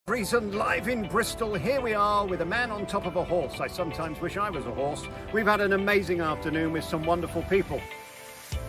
Alex Belfield... 🐎 "man on top of a HORSE, I sometimes WISH I was a HORSE..." 🐎
This is uploaded so people can learn what Alex is really like. This video will be deleted when Alex is sentenced in September, so victims can move on with their lives. Age restricted as not suitable for all viewers.

Alex creates these sorts of videos (this is a small clip from a long video) and then emails and tweets all known friends, colleagues and family of the person he is ranting on about. On many occasions he states a continuous action, saying he isn't going away.